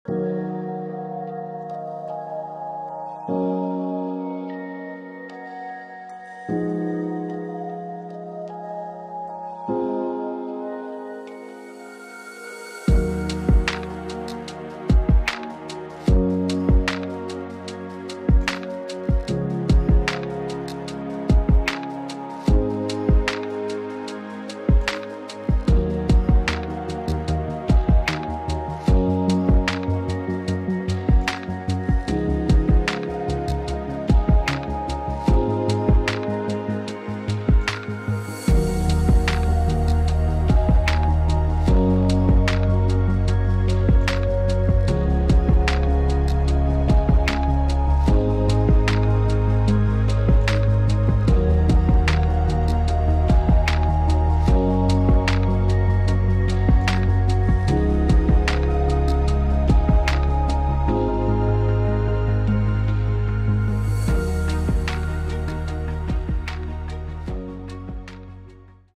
Retreat at Rum Point 25 | SOLD! | Cayman Kai | Cayman Islands Sotheby's International Realty
Spectacular, unobstructed North Westerly facing views of sunset and sunrise. Immaculately maintained, ground floor, end unit offers great value for money. An opportunity like this rarely comes on the market.